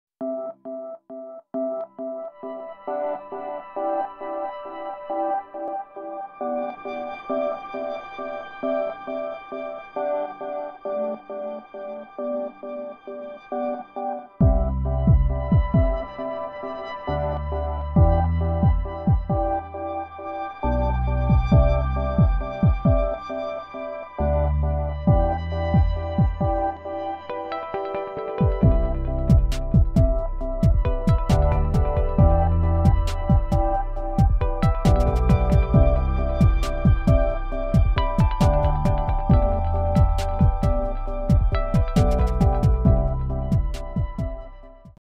Dlouhozobka svízelová | Hummingbird hawk-moth (motýl kolibřík - hummingbird butterfly)
Dlouhozobka svízelová je motýl, kterého si však lidé často pletou s kolibříkem. Je nádherná!

The Hummingbird hawk-moth is a butterfly that people often confuse with the hummingbird. It's beautiful!

Arigatou gozaimasu!